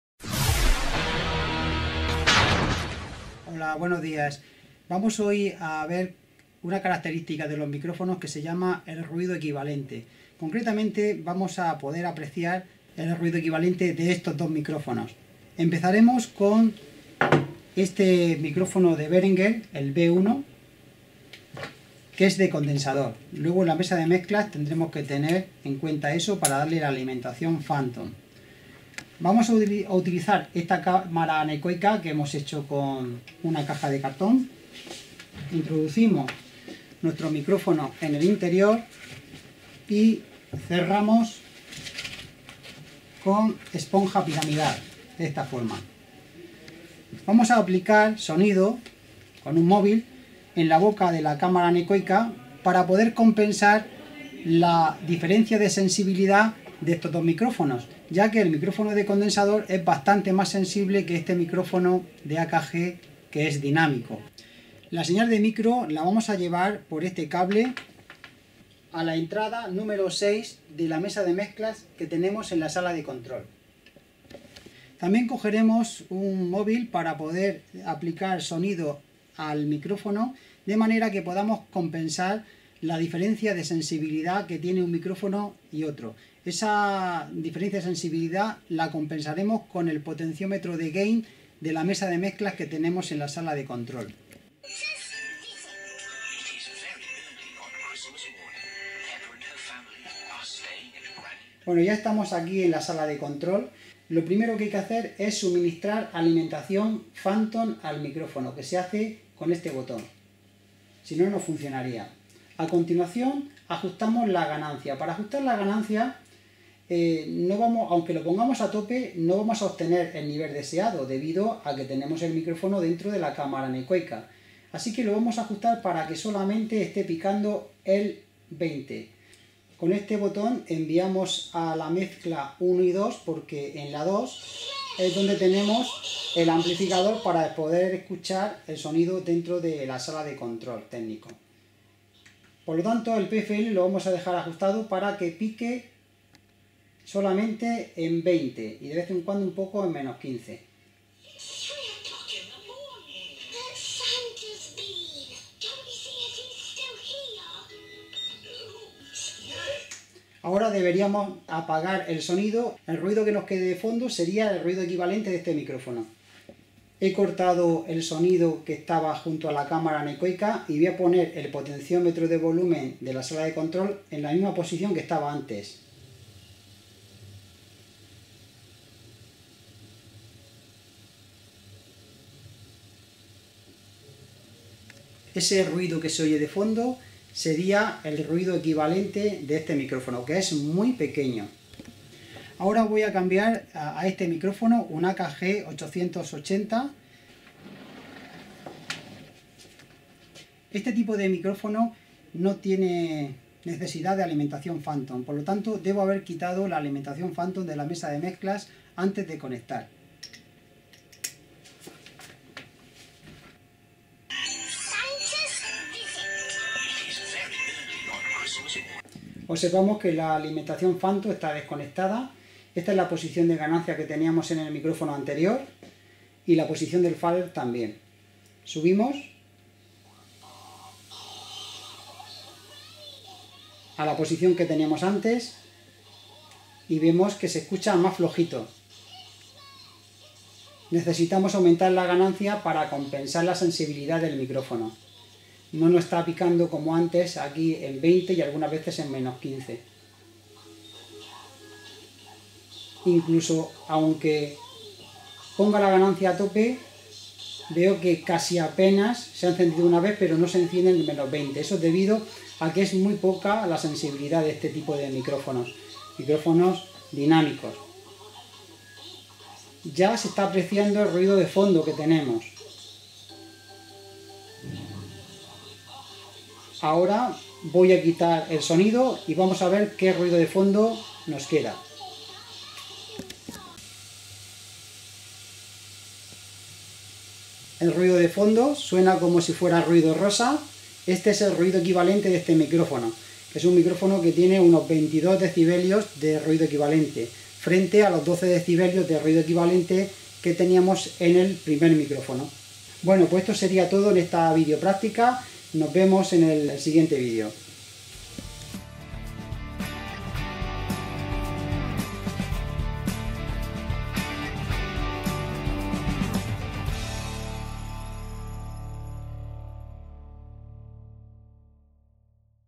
MICRÓFONOS Ruido equivalente de un micrófono
Mi Blog:   El Cajón del Electrónico

En esta videopráctica, veremos una característica fundamental de todo micrófono: EL RUIDO EQUIVALENTE.
El ruido equivalente es el ruido que entrega un micrófono aunque no esté captando ningún sonido. Se mide en decibelios y lo ideal es que su valor fuera 0 dB pero es difícil encontrar micros asequibles con un nivel de ruido equivalente menor de 10 o 12 dB.
 Para entender mejor su significado pondré un ejemplo.

Un ruido equivalente de 22 dB en un micrófono indica que el micrófono  entrega un nivel de ruido igual a la señal que entregaría si estuviera captando un sonido de 22 dB. Como podemos suponer, cuando menor sea el ruido equivalente mejor será el micrófono y nos permitirá grabar sonidos más débiles.

En esta videopráctica analizamos dos micrófonos bien distintos:

-  Un Behringer B1, de condensador, con una sensibilidad alta de 20 mV/pa y un ruido equivalente de 13 dB

-  Y un AKG D880, dinámico, con una sensibilidad baja de 2,2 mV/pa y un ruido equivalente de 22 dB

Para intentar equiparar el nivel de PFL de ambos micros, corregimos el potenciómetro de Gain de la mesa de mezclas, aunque es tanta la diferencia de sensibilidad que no lo conseguimos.

Es importante para hacer la comparación de ruido equivalente que se equipare el nivel de PFL en la mesa de mezclas, ya que en caso contrario se estaría falseando la comparativa. En nuestro caso el nivel de PFL del micro AKG ha sido algo menor que el Behringer, pero aún así se ha apreciado un ruido equivalente muy superior en el AKG.

Como conclusión, cuando compremos un micrófono debemos fijarnos el nivel de ruido equivalente que posee. Valores por debajo de 15 dB no indican que tenemos un micro de calidad. Si el valor sobrepasa los 25 dB, tenemos que tener presente que va ha tener un ruido de fondo que posiblemente nos resulte molesto y no vamos a poder hacer grabaciones de bajo nivel sonoro.

UN SALUDO.